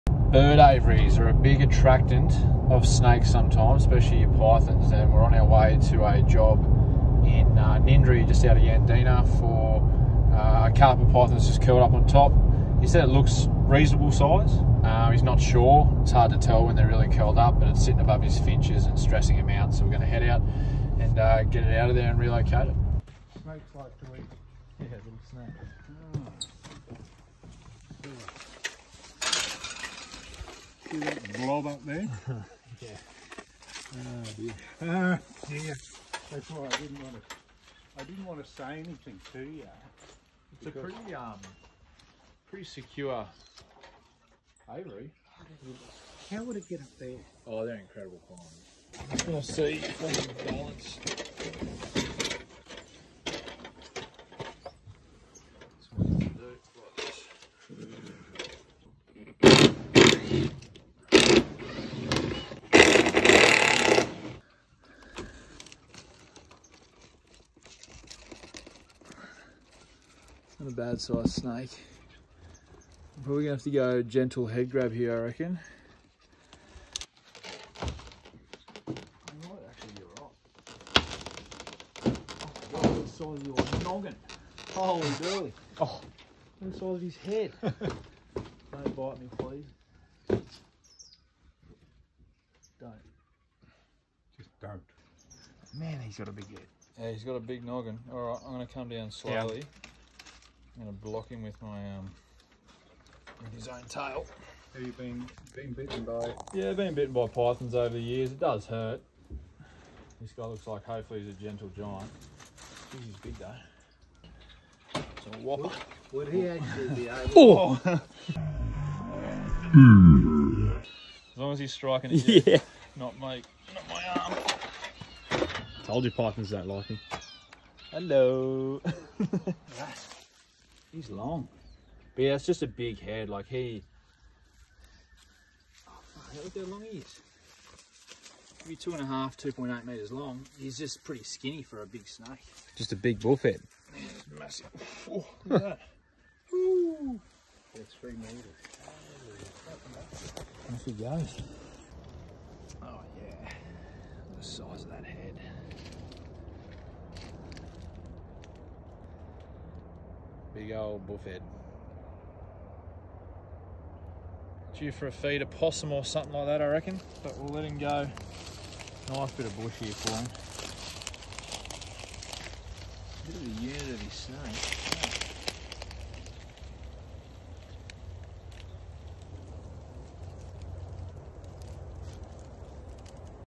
Big Boof Headed Python On Bird Aviary! 🐦
A gentleman in Ninderry got a surprise when he went down to feed his finches this afternoon. The big snake was curled up in a perfect coil and was a bit bigger than I thought when I first pulled him out from on top of the aviary.

The carpet python would have been about 2.7 meters long but a little skinny for its length but gee whiz didnt it have a big head! This bird aviary was one of the most snake proof aviaries I have ever seen and this snake was no chance of getting in, however it had to be relocated as the snake was making the birds nervous.

- Stu & Westy